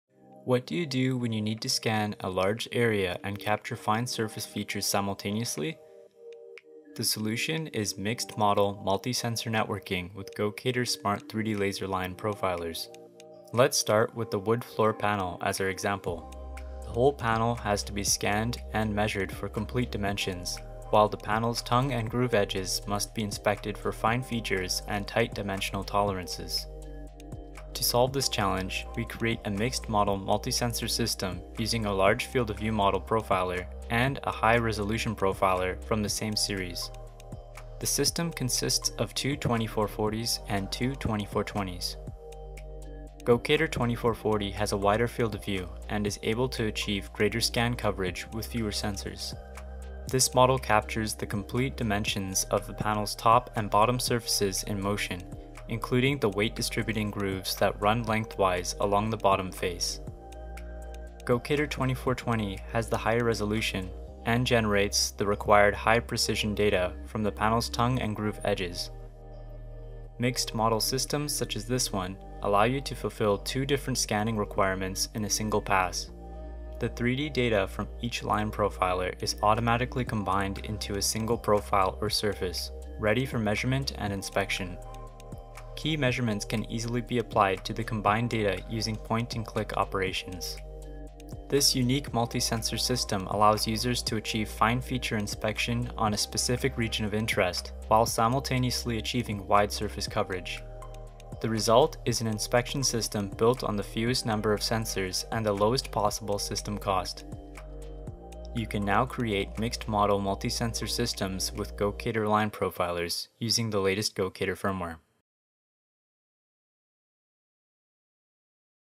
Gocator Mixed Multi Sensor Networking
Gocator now supports mixed multi sensor networking, allowing users to mix and match sensors from the same series. Here we explore an example of scanning a floorboard, where half of the inspection task requires fine feature measurement, and the other half requires large a large FOV.

0:00 Intro
0:13 Inspection Challenge
0:37 Large Feature Inspection
1:00 Fine Feature Inspection
1:16 Combined Data
1:30 Conclusion